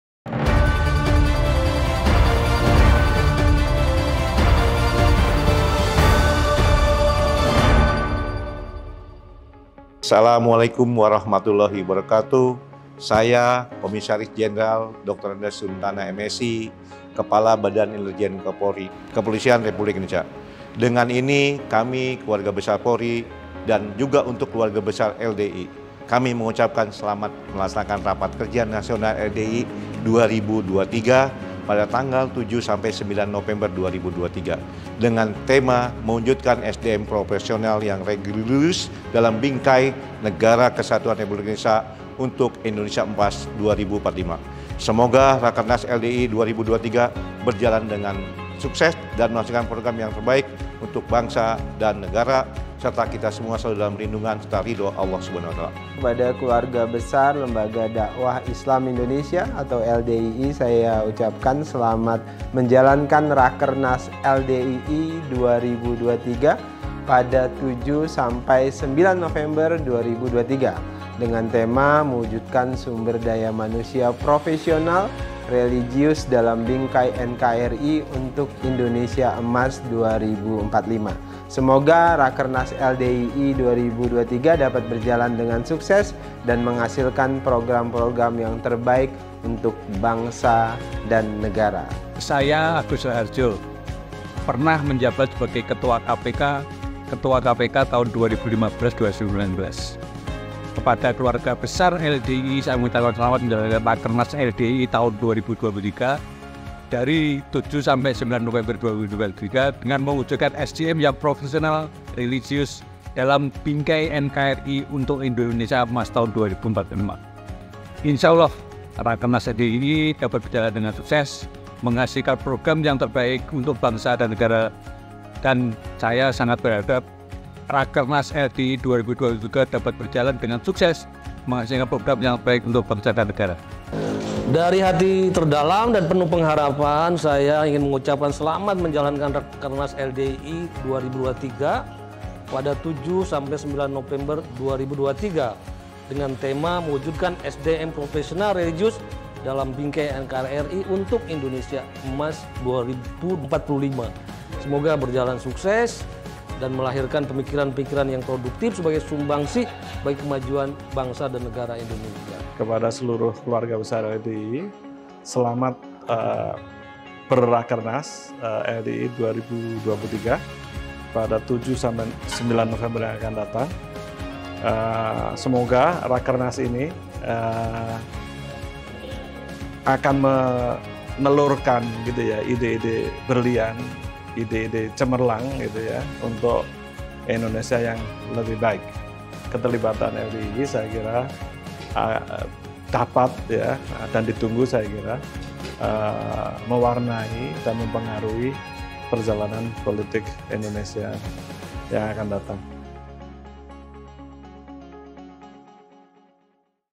DUKUNGAN TOKOH-TOKOH NASIONAL UNTUK RAKERNAS LDII 2023 #2
DPP LDII akan menggelar Rapat Kerja Nasional (Rakernas) pada 7-9 November. Acara tersebut akan dibuka Presiden Joko Widodo dan dihadiri tiga calon presiden, yang akan memberi pembekalan dalam acara Rakernas. Rakernas dihelat di Grand Ball Room Ponpes Minhajurrsoyiddin Jakarta.

Rakernas merupakan perhelatan akbar LDII yang merupakan agenda tetap untuk menajamkan program kerja yang dibuat saat musyawarah nasional. Dalam Rakernas nanti, LDII akan mempertajam sekaligus mengevaluasi program kerja “8 Bidang Pengabdian LDII untuk Bangsa”.

Berikut dukungan tokoh-tokoh untuk Rakernas LDII 2023.

Facebook : Lembaga Dakwah Islam Indonesia